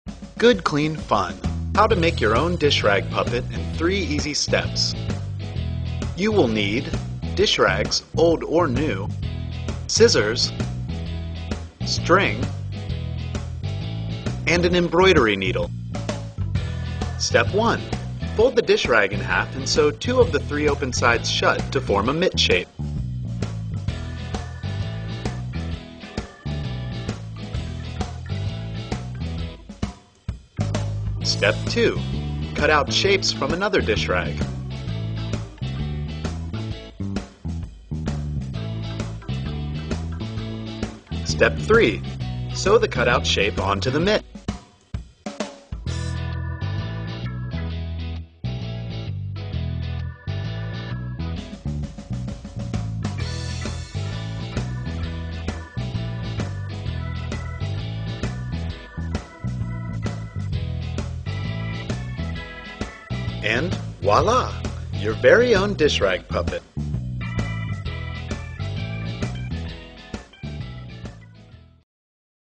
How to Make Dish Rag Puppets
Good Clean Fun!
Materials: Washcloths or dishrags,
old/used, clean Scissors
String
Embroidery needle
Instructions:
1.fold dishrag/washcloth in half and sew two of the three open sides shut to form a mitt shape.
2.Cut shapes etc. out of another old washcloth/dishrag and sew them onto the mitt
3.Note: you can sew design onto half of dishrag before you sew the mitt shut for easy maneuvering
4.Note: you can use fabric glue instead of needle and string if you're not a sewer